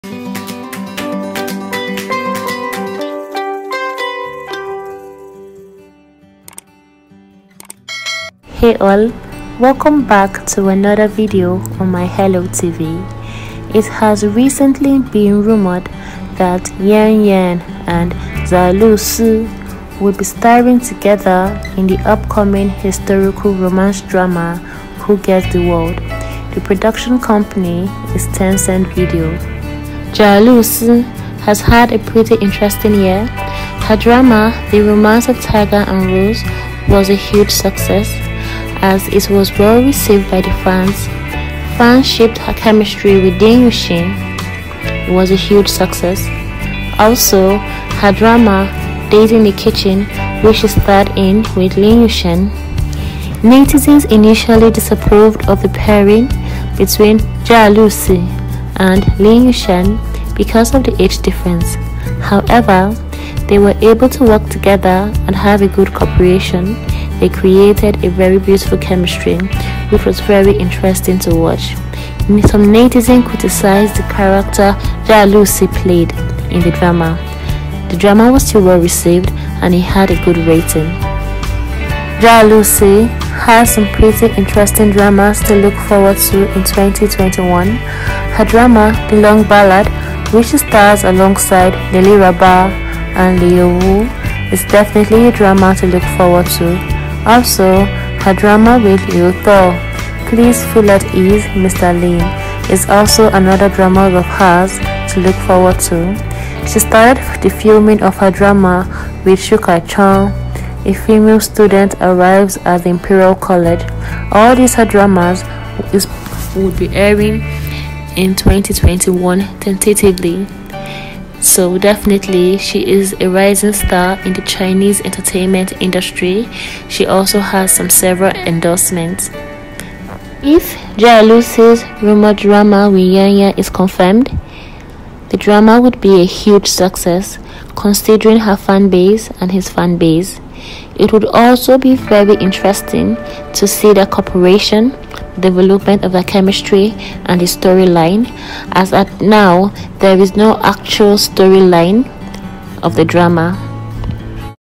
Zhao Lusi & Yang Yang Who Rules the World
Hey All welcome to another video on my channel.In this video I will talk about Yang Yang and Zhao Lusi l drama